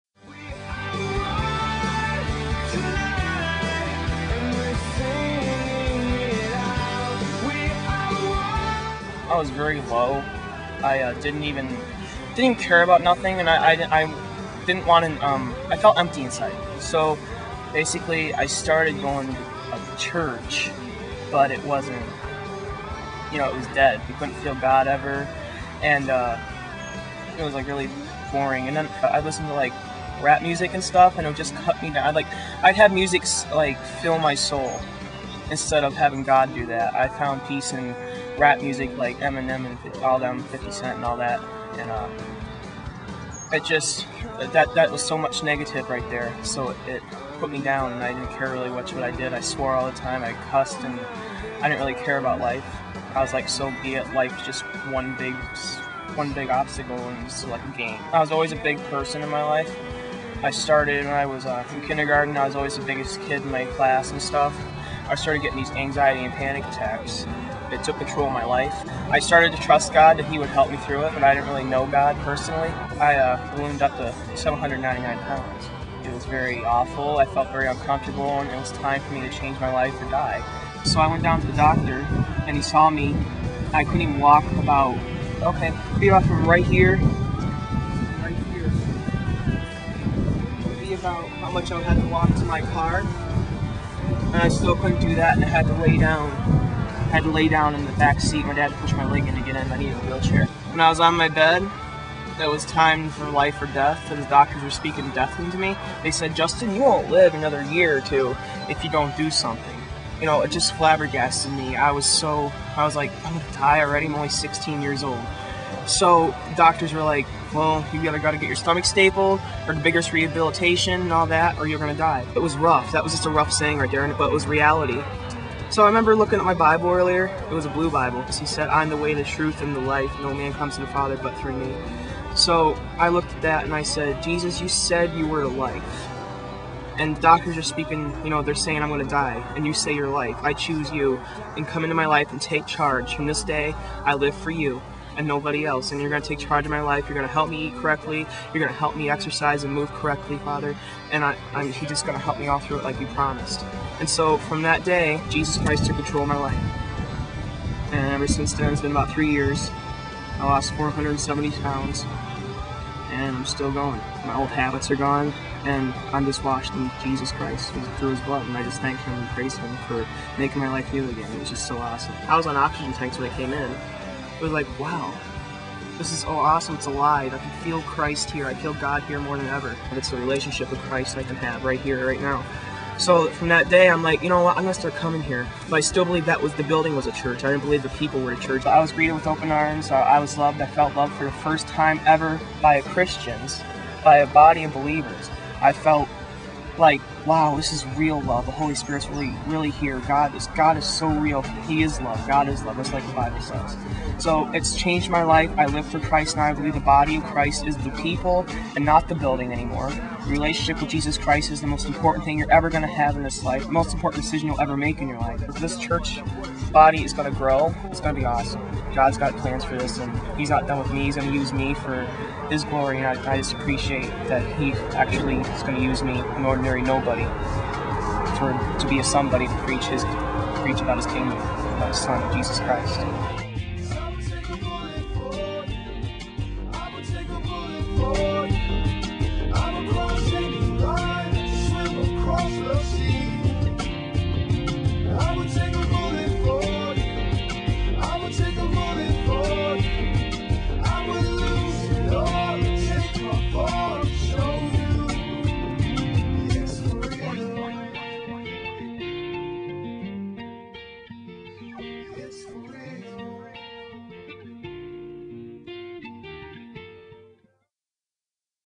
Vision: Justin Willoughby Open Arms Church
Justin Talks about how he lost almost 500 pounds.